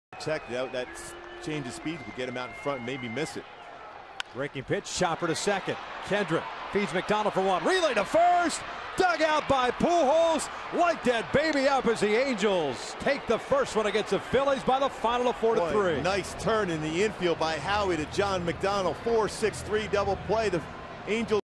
LAA@PHI: Frieri gets double play to end the game
5/13/14: Ernesto Frieri gets Carlos Ruiz to ground into a 4-6-3 double play to give the Angels a 4-3 win and earn his fifth save